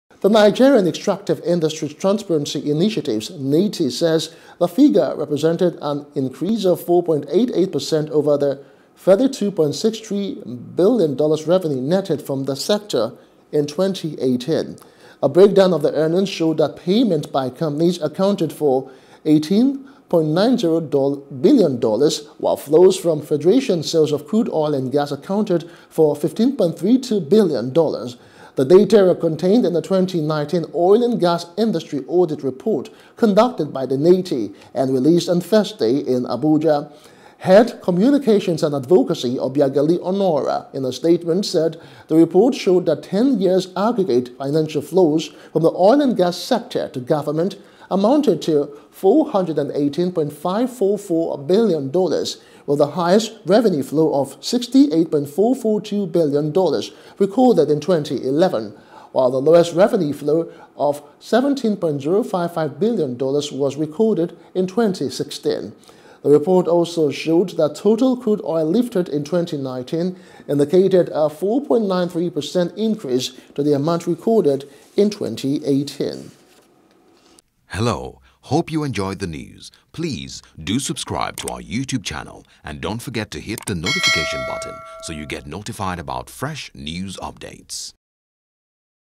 Oil And Gas ‘Nigeria Earned $34.22B In 2019’ – NEITI | BUSINESS
The Nigerian Extractive Industries Transparency Initiatives (NEITI) said

the figure represented an increase of 4.88 per cent over the $32.63 billion revenue netted from the sector in 2018.